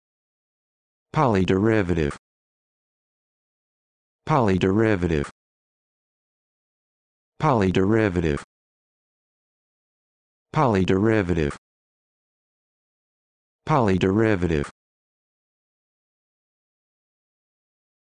How to Pronounce Polyderivative
This video shows you how to pronounce Polyderivative